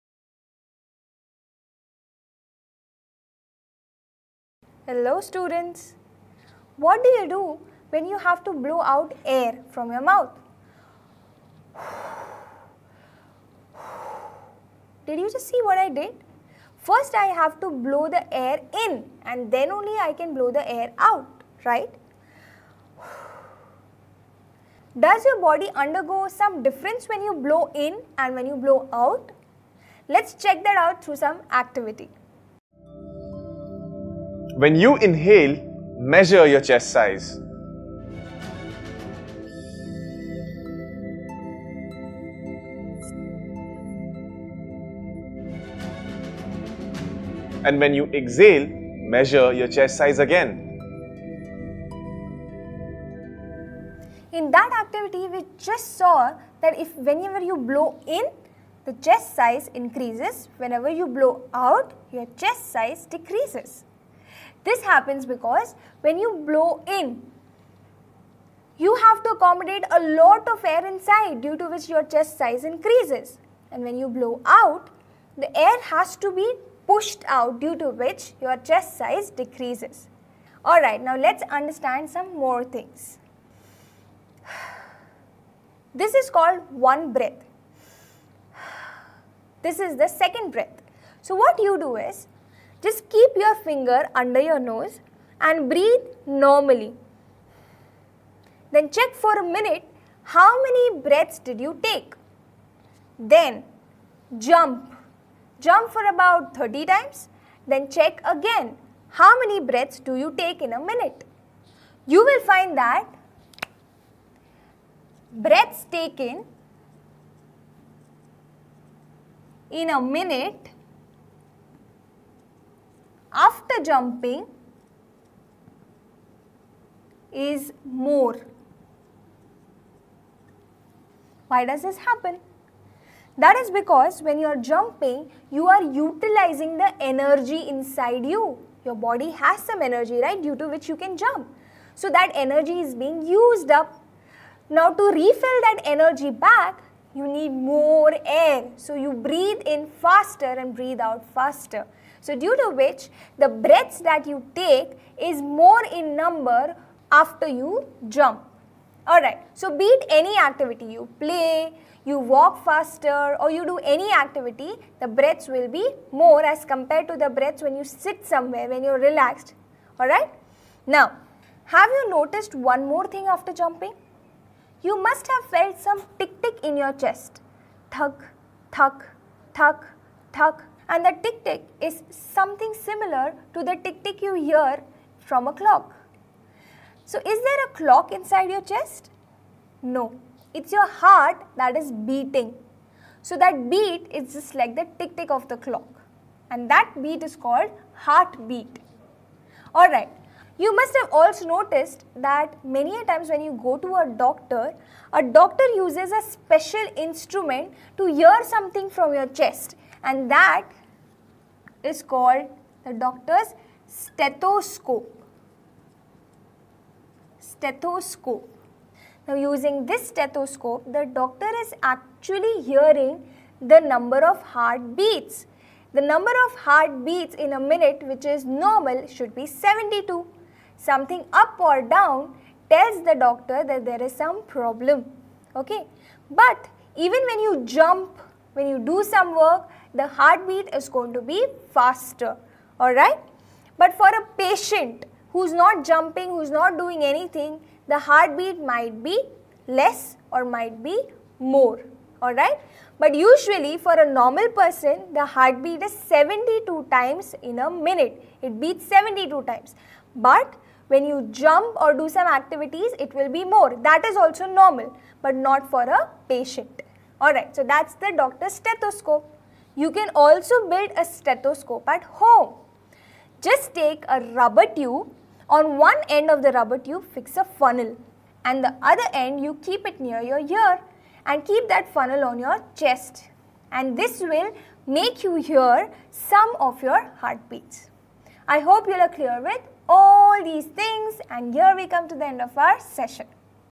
What Is Respiration? | Blowing In and Out | Class 5 EVS (CBSE, NCERT)
As you are reading this, you are simultaneously breathing in oxygen and giving out carbon dioxide. Did you realize this? This is an action that you do continuously. To put it in scientific terms, it is a process called respiration that is happening. Let us explore this life process.

* 2000+ hours of bite-sized video lectures